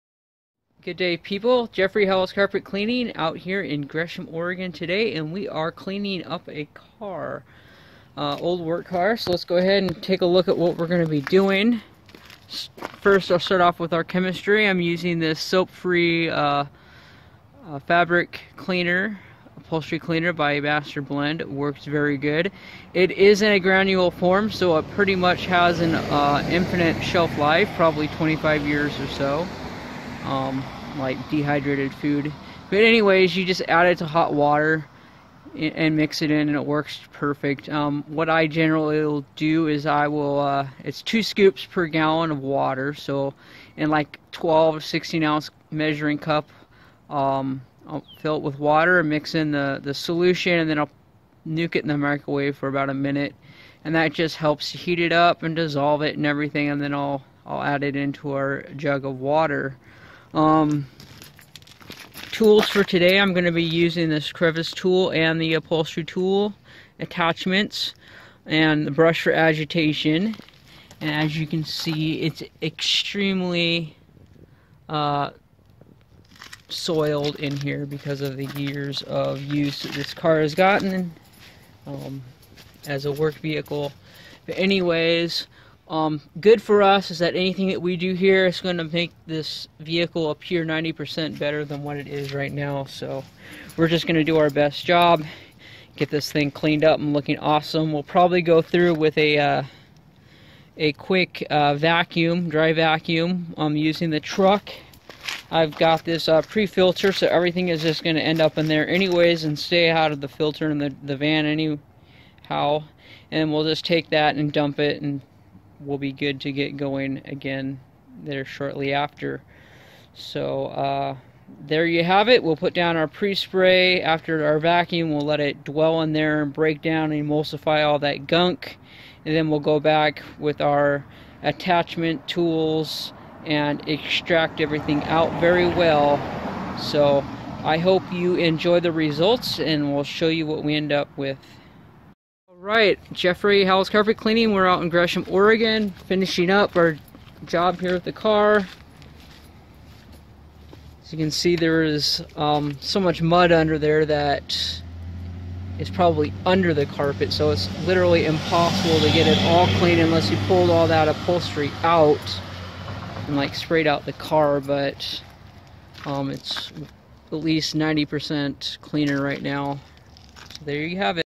Vehicle Upholstery Cleaning Gresham
Steam Cleaning & Vehicle Upholstery Cleaning Gresham. We remove pet urine stains.

This video displays basic vehicle upholstery cleaning in Gresham, Oregon. The chemistry is a soap less fabric/upholstery cleaning solution in granular form my Masterblend. The upholstery was treated and extracted twice. The front passenger seat area of the car displayed excessive soiling under the upholstery that was unable to be extracted short of ripping the upholstery out of the car.